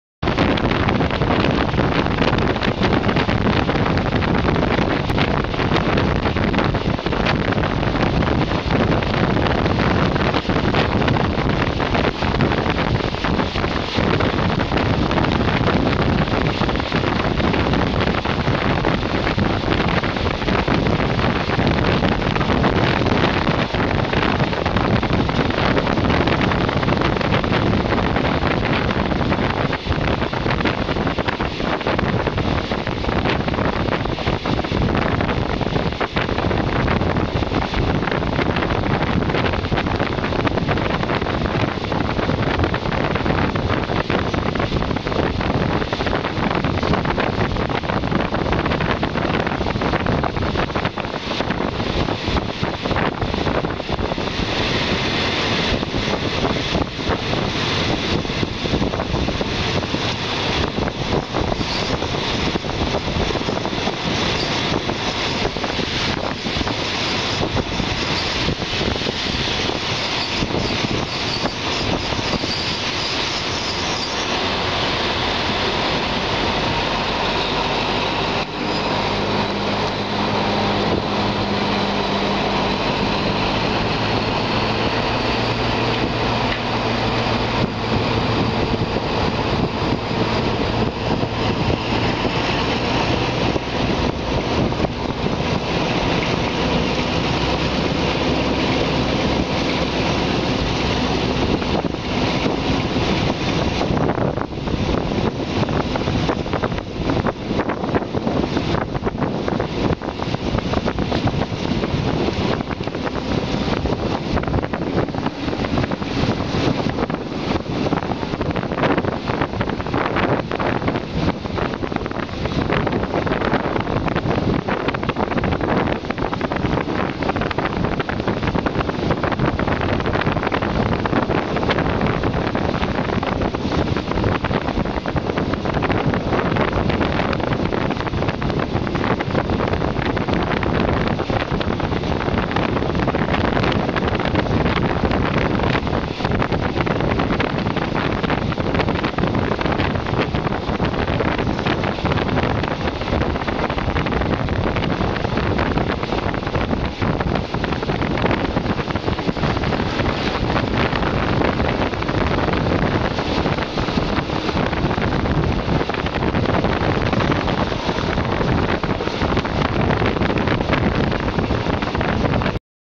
รถไฟ-283 ชลบุรี
ขบวนรถไฟที่ 283 กรุงเทพ - บ้านพลูตาหลวง ที่สถานีรถไฟชลบุรี